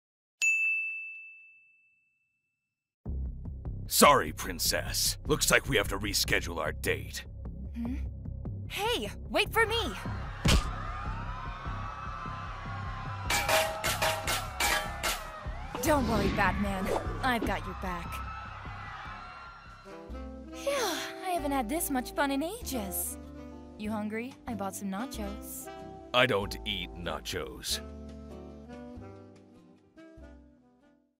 Batman X Wonder Woman: Fight Night | Bonny comic dub
Nobody is getting in the way of their date.